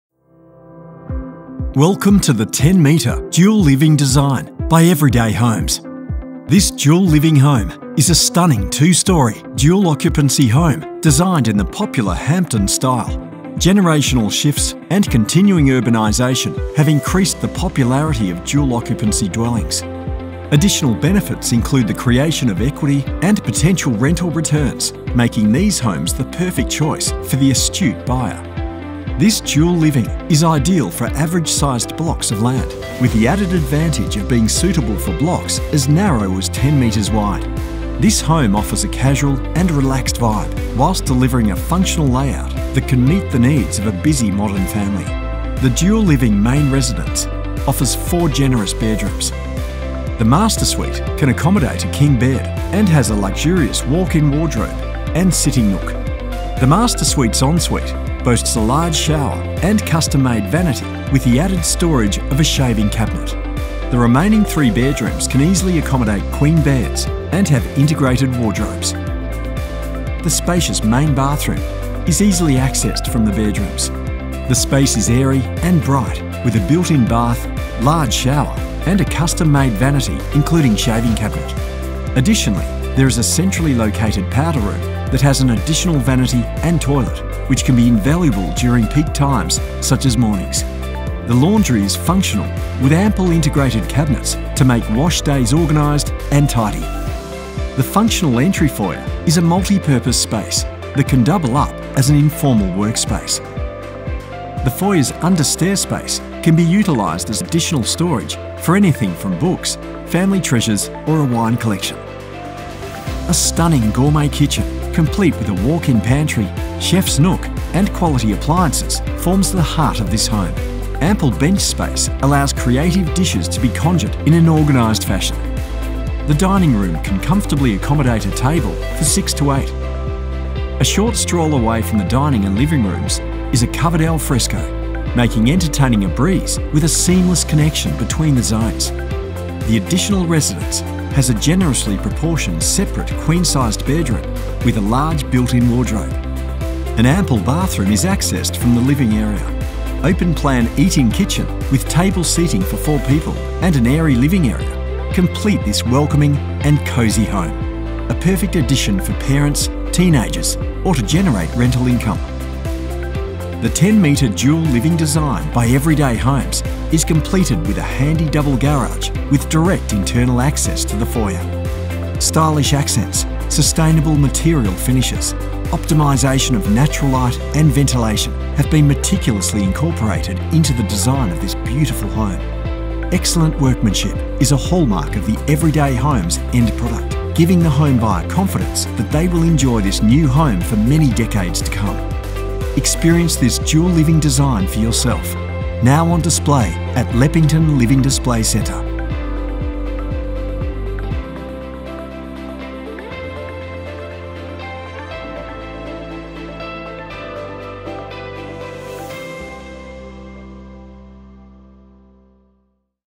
10m Dual Living Design
Our Double Storey 10m Dual Living Design incorporating a Granny Flat is now on display at Leppington Living.